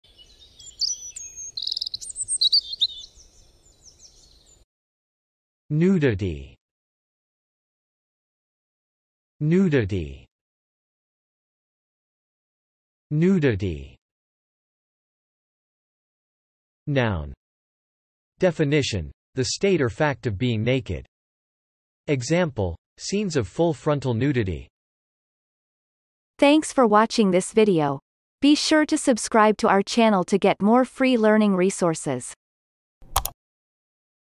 How to pronounce nudity - Vocab Today
The pronunciation of the word nudity in Amercian accent is demonstrated in this video. If the word is from another language, such as brand name, it will be pronounced in its original language. Based on the word frequency, this is of 3 stars difficulty.

The word nudity has the following meanings:
1. (noun)
Definition : The state or fact of being naked.
Example : scenes of full frontal nudity